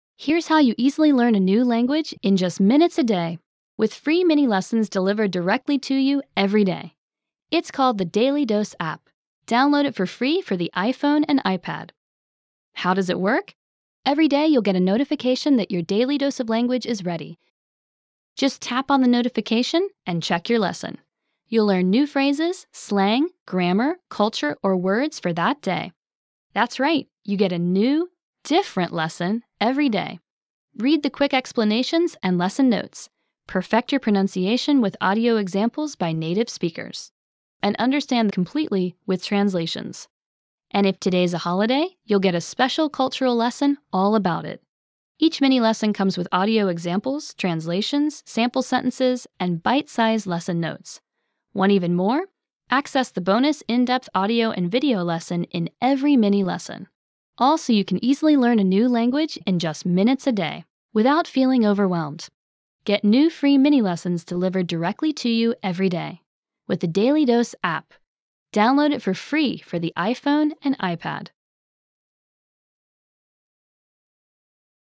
Get Language Mini-Lessons Delivered To You Everyday!
Want to easily learn a language in minutes a day? You’ll need a daily dose of free mini-lessons. And this is exactly what you get with our FREE new App, the Daily Dose of Language for the iPhone and iPad! Every day, you’ll get a QUICK new lesson covering phrases, slang, grammar, culture or words for that day. Learn a language in just minutes a day, every day, without feeling overwhelmed.